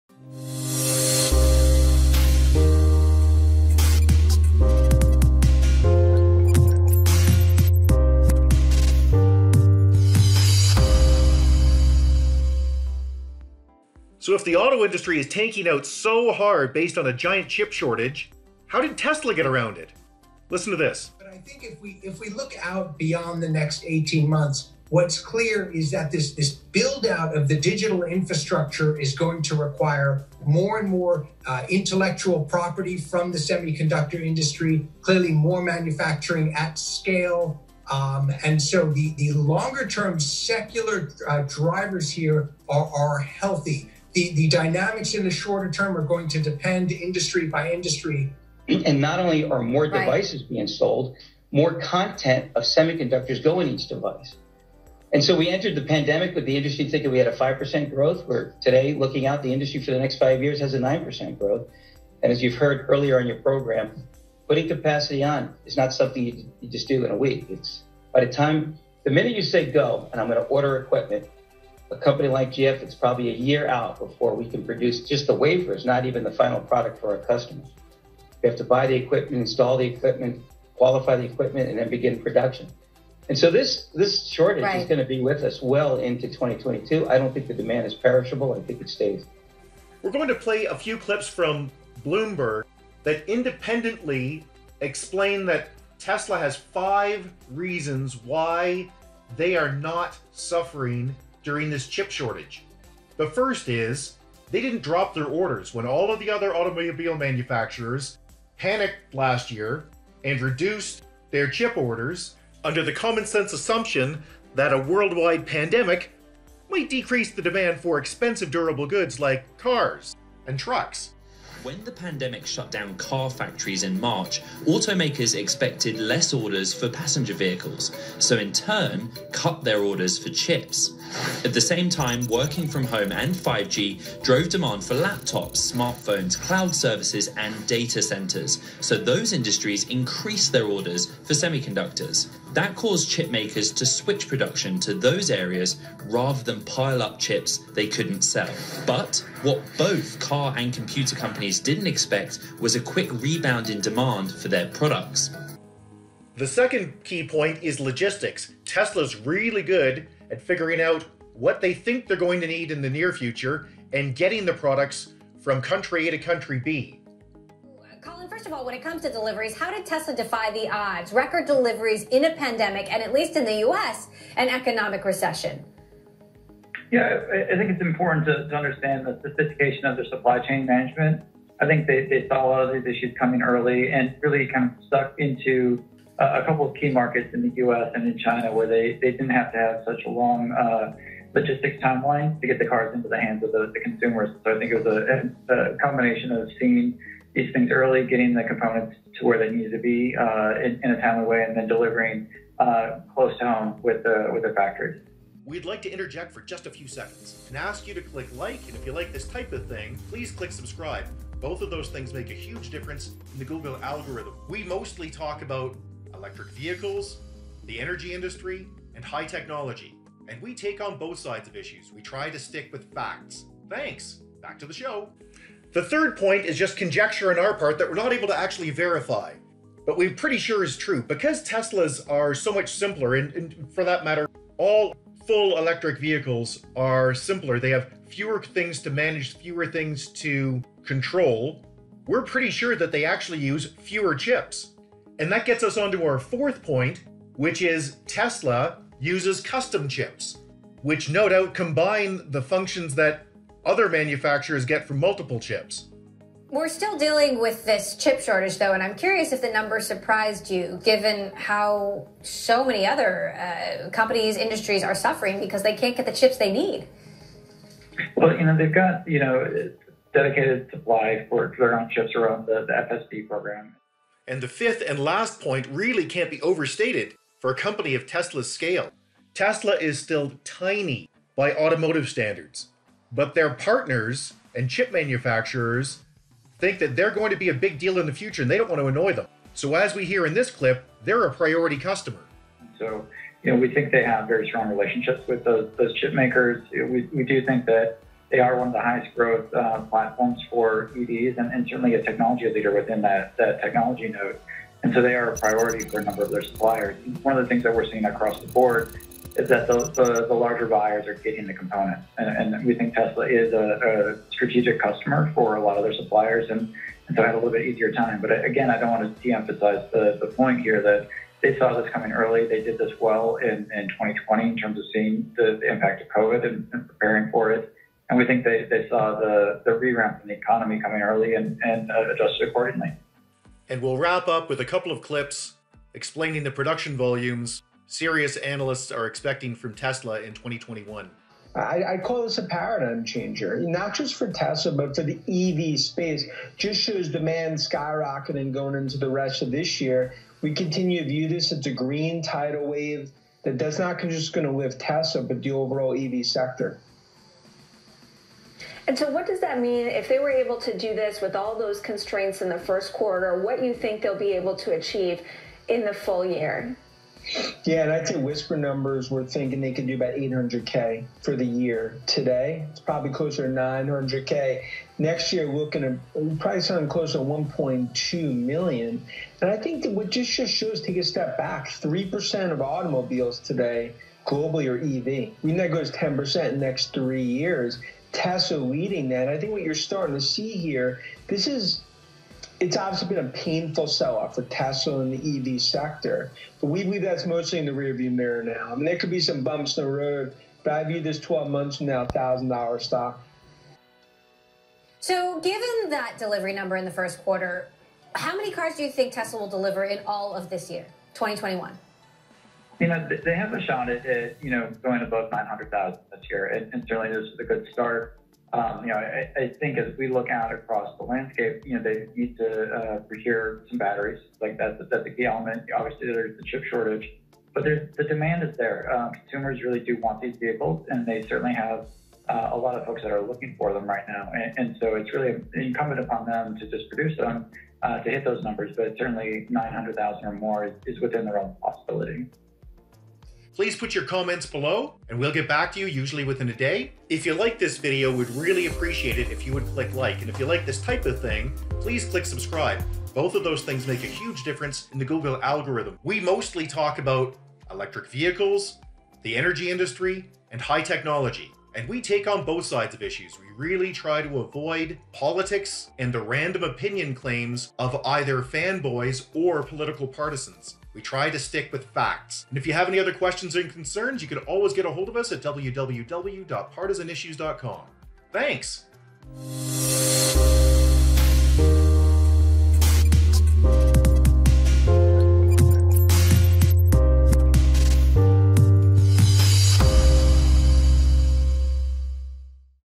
How Tesla Avoided the Global Chip Shortage
If you follow the tech or auto industry you know about the massive computer chip shortage.  That chip shortage is costing the auto industry billions of dollars because hundreds of thousands of cars and trucks can not be produced without these key components.  Well, that is true for Ford, Chrysler, GM, Honda and Toyota, it is not true for Tesla.  So how did Tesla avoid the chip shortage.

Introduction 0:00
Global Chip Shortage Explained 0:20
1: Tesla did not decrease orders at the start of the pandemic 1:34
2: Tesla has the best logistics in the business 2:36
3: Tesla vehicles use fewer chips 3:45
4: Telsa uses custom chips 4:06
5: Tesla is a priority customer to the silicon chip industry 4:34
Tesla Stock Predictions 5:33

Dan Ives of Wedbush Securities and Colin Rusch from Oppenheimer are highly featured.